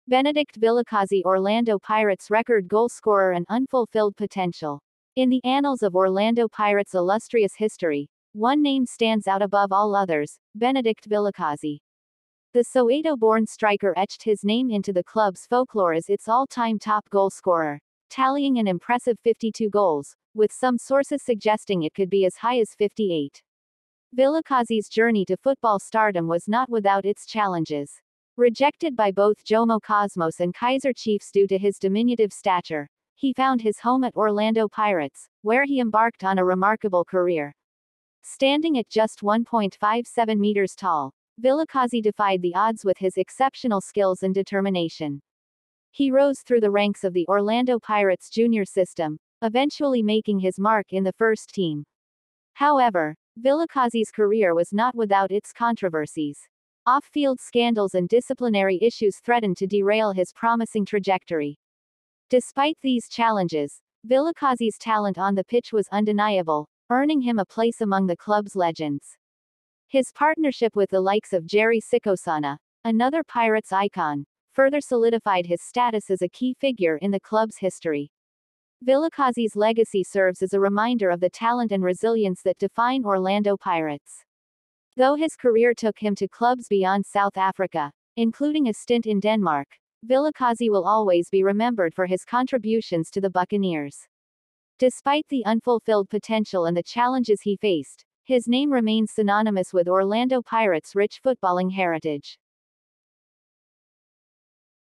Benedict Vilikazi: Orlando Pirates' Record Goal Scorer and Unfulfilled Potential
Title: The Legend of Benedict Vilikazi: Orlando Pirates' Record Goal Scorer

Description:
🏆 Witness the remarkable tale of Benedict Vilikazi, the Soweto-born sensation who etched his name into the annals of football history as Orlando Pirates' all-time top goal scorer! 🌟 Standing at just 1.57 meters tall, Vilikazi defied the odds with his extraordinary skills and unwavering determination. Join us as we delve into his captivating journey from rejection to redemption, from the streets of Diepkloof to the hallowed grounds of Mayfair. 💥 But Vilikazi's path to glory was not without its challenges - scandals, disciplinary issues, and the weight of expectations threatened to derail his rise to stardom. Yet, through it all, his talent shone bright, leaving an indelible mark on the beautiful game. ⚽️ From rubbing shoulders with icons to battling adversity on and off the pitch, Vilikazi's story is one of triumph and resilience. So join us as we celebrate the legacy of a true footballing hero, whose name will forever be synonymous with the pride and passion of Orlando Pirates! 🔥